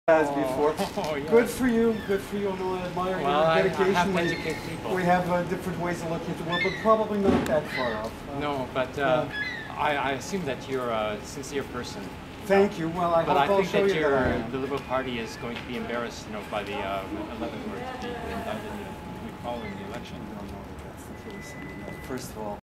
Élection 2014 - abraham Weizfeld - PVQ D'Arcy-McGee 9/14
Debate with Liberal Party candidate David Birnbaum